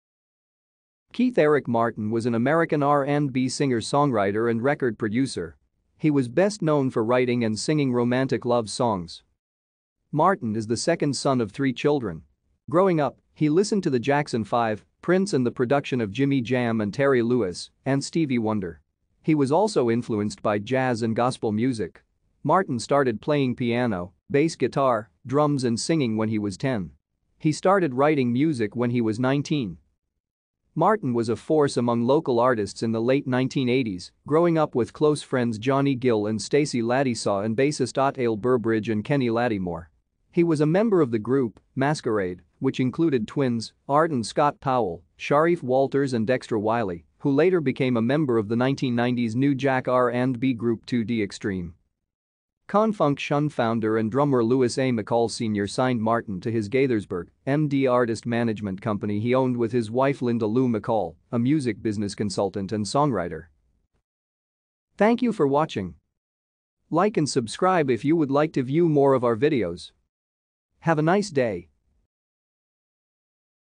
Keith Martin (Musician)
Keith Eric Martin  was an American R&B singer-songwriter and record producer. He was best known for writing and singing romantic love songs.